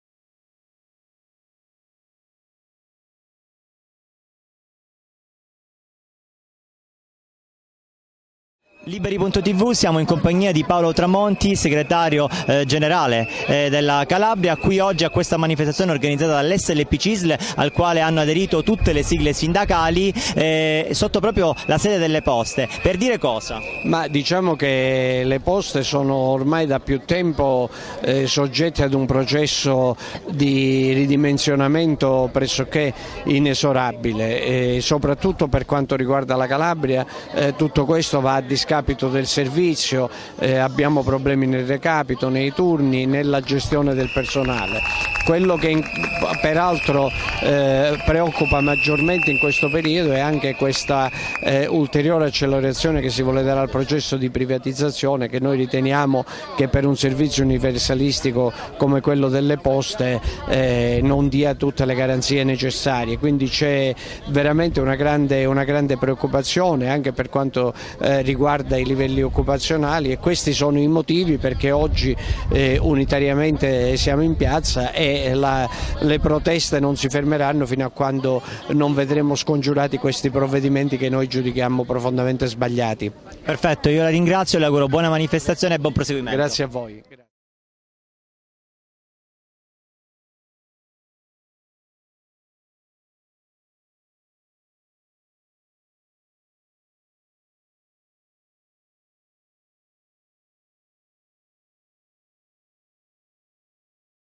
Manifestazione 18 Luglio 2016 - Intervista Paolo Tramonti Segretario Generale SLP Cisl Calabria
Manifestazione regionale unitaria lavoratori Poste Italiane SPA.
Catanzaro, 18 luglio 2016, sit-in di protesta in piazza L. Rossi.
Intervista aPaolo Tramonti Segretario Generale SLP (Sindacato dei Lavoratori Poste) Cisl Calabria.